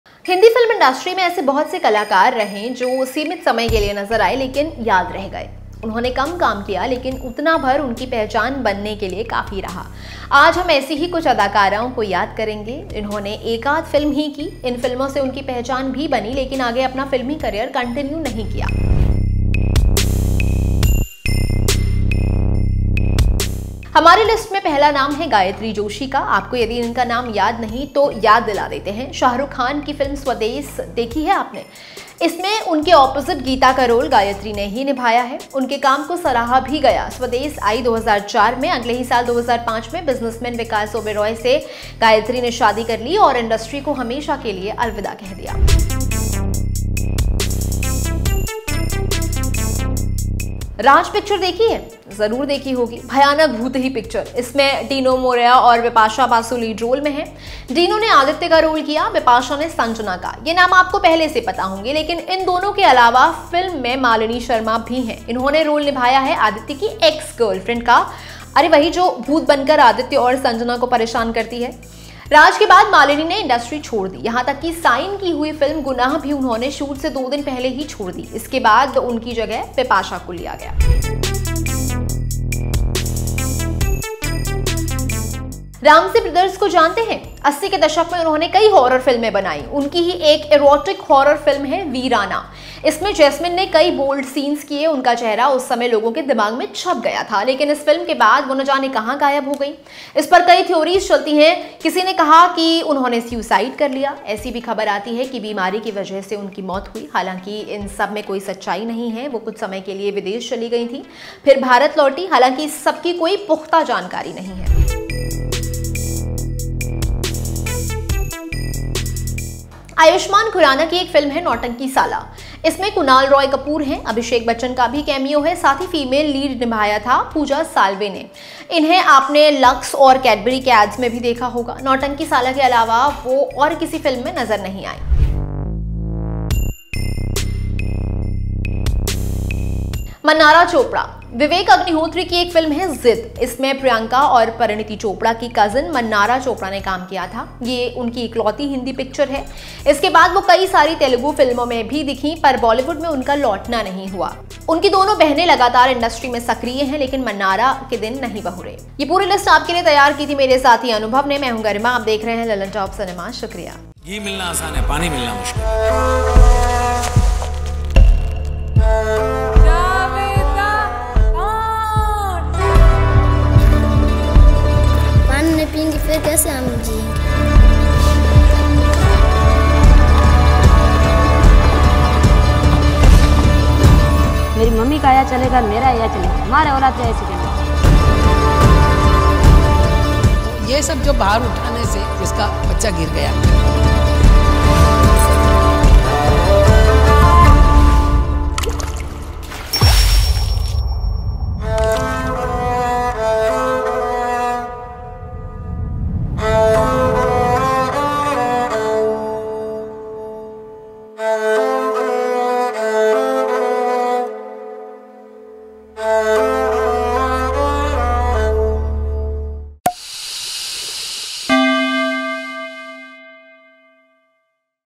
Shah Rukh Khan की फिल्म से डेब्यू करने वाली ऐक्ट्रेस भी लिस्ट में, एक के सुसाइड की अफवाह उड़ी
In the Hindi film industry, there have been numerous actors who made a lasting impact despite appearing for a limited time on screen. Their memorable performances in just a few films were enough to establish their identity. Today, we will pay talk about some such actresses who gained recognition through a single movie. Although they didn't pursue their film careers further, their legacy was etched in the hearts of audiences forever.